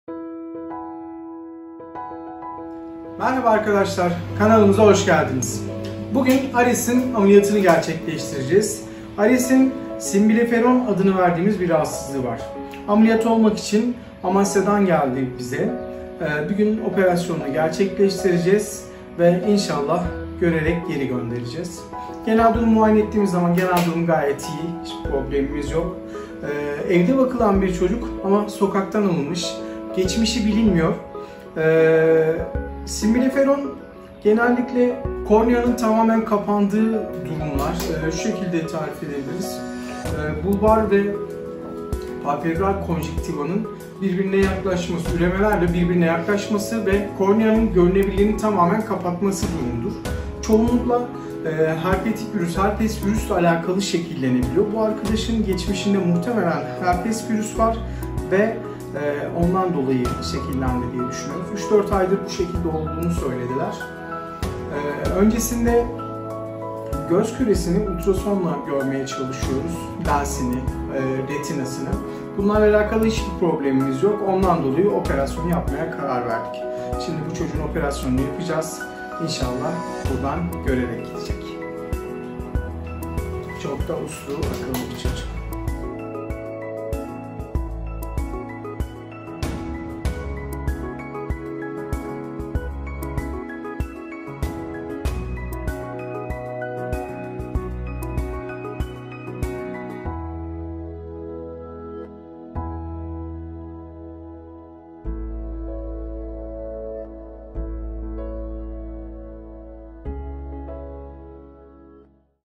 Kedi'de Semblefaron Operasyonu. Uzun Zamandır Göremeyen Kedi Görebilecek Mi ?
Gözü çevreleyen konjonktiva tabakası ile göz kapaklarını içten çevreleyen konjonktiva tabakalarının değişik etkenler neticesinde birbirine yapışmasıdır.
(Glob ve göz kapakları arasında yapışıklık oluşmasıdır.)
Gözde ve kapaklarda hareket kısıtlılığına ve fonksiyon bozukluğuna sebep olabilir.